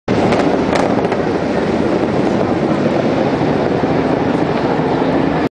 Репетиция парада. Генеральный прогон от Центрального телеграфа. Авиация над Тверской. Съемки во время репетиции парада. Истребители над Тверской. Дальние бомбардировщики. Танки в городе. Дозаправка в воздухе. Найди себя в кадре.